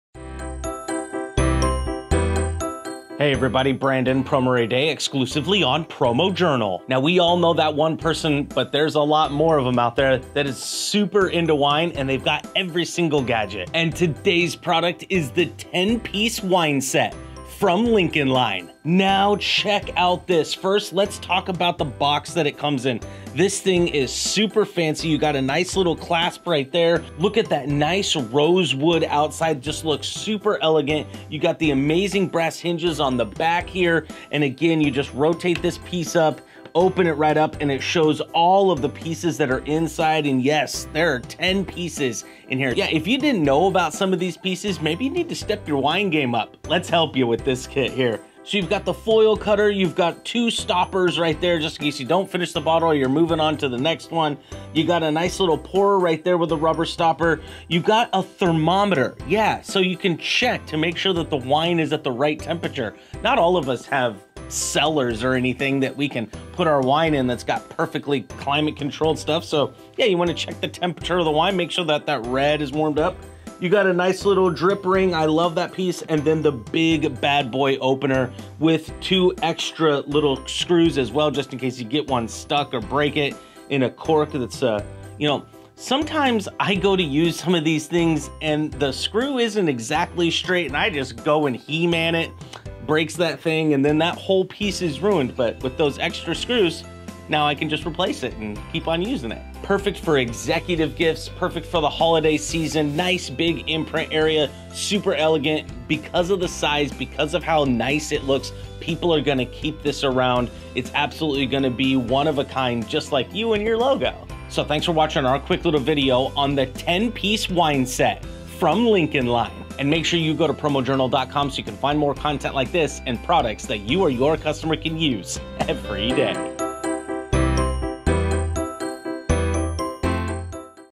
10pc Wine Set (Item WNE900)
This 10-pc wine set includes a lever corkscrew with 2 replaceable corkscrews, 2 bottle stoppers, foil cutter, drip collar, aerator and thermometer. The wine tools come packed in a hinged box with latch closure.